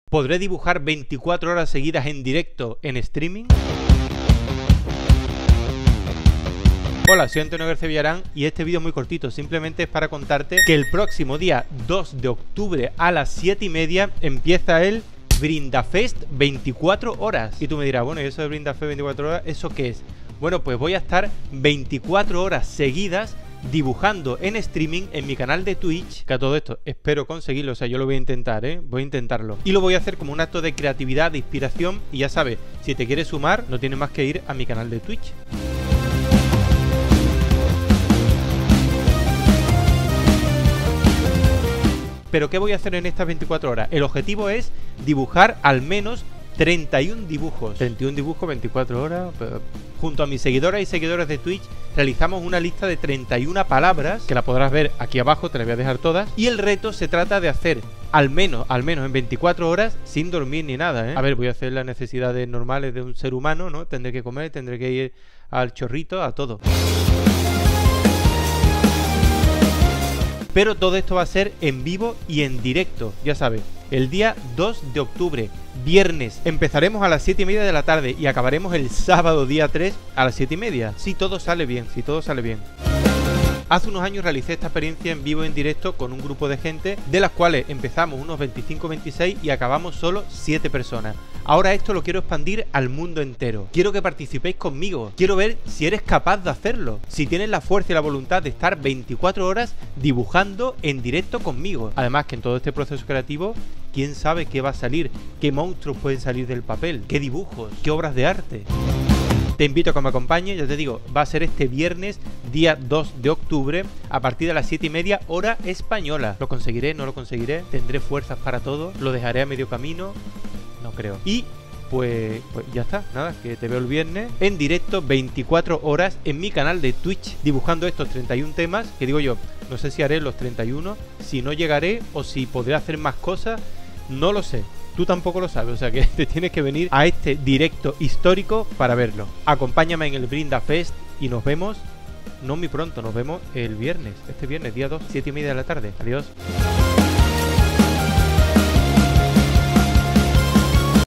¿24 horas dibujando en directo? ¿Lo conseguiré? VrindaFest 24 Horas
VrindaFest 24 Horas -Dibujando en vivo - Antonio García Villarán-
Del viernes 2 a las 19,30 horas al sábado 3 de octubre a las 19,30 horas, 2020. Lugar: Mi canal de Twitch;
¡Participa conmigo en esta iniciativa creativa y artística!
Dibujaremos 31 motivos basándonos en 31 palabras:
1- Llave
2- Muerte
3- Mantis
4- Emocional
5- Esencia
6- Esclavitud
7- Pandemia
8- Gula
9- Escrúpulo
10- Deriva
11- Yokai
12- Lúgubre
13- Sacrificio
14- Leopardo
15- Pozo
16- Equilibrio
17- Efímero
18- Sarcófago
19- Refugio
20- Sacrilegio
21- Piel
22- Simetría
23- Sombra
24- Bizarro
25- Proscrito
26- Caos
27- Divinidad
28- Conceptual
29- Ego
30- Furia
31- Etéreo

Únete al reto. Comparte esta publicación y nos vemos el viernes.